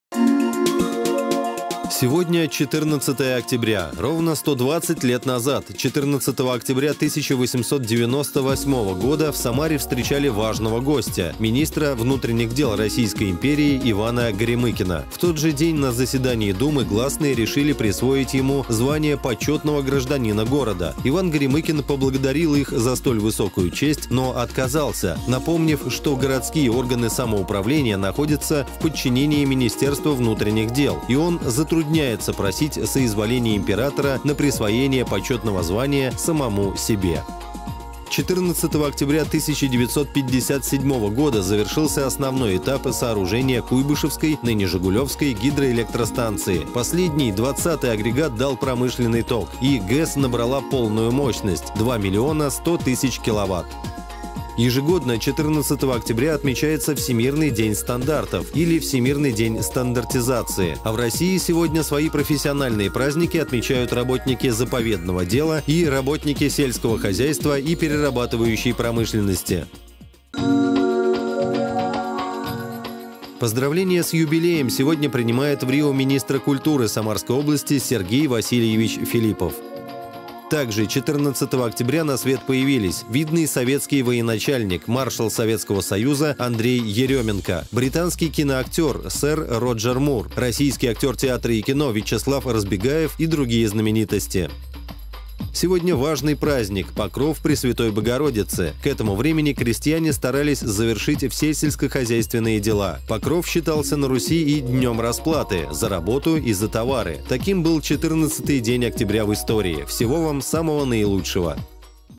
Календарь губернии от 14.10.2018
Ровно 120 лет назад, 14 октября 1898 года, в Самаре встречали важного гостя - министра внутренних дел Российской империи Ивана Горемыкина. В тот же день на заседании думы гласные решили присвоить ему звание почётного гражданина города. Иван Горемыкин поблагодарил их за столь высокую честь, но отказался, напомнив, что городские органы самоуправления находятся в подчинении министерства внутренних дел, и он затрудняется просить соизволения императора на присвоение почётного звания самому себе.

14 октября 1957 года завершился основной этап сооружения Куйбышевской (ныне - Жигулёвской) гидроэлектростанции. Последний агрегат дал промышленный ток и ГЭС набрала полную мощность - 2 млн 100 тысяч киловатт.
Ежегодно 14-го октября отмечается Всемирный день стандартов, или Всемирный день стандартизации. А в России сегодня свои профессиональные праздники отмечают работники заповедного дела и работники сельского хозяйства и перерабатывающей промышленности.
Сегодня важный праздник - Покров Пресвятой Богородицы. К этому времени крестьяне старались завершить все сельскохозяйственные дела. Покров считался на Руси днём расплаты за работу и за товары.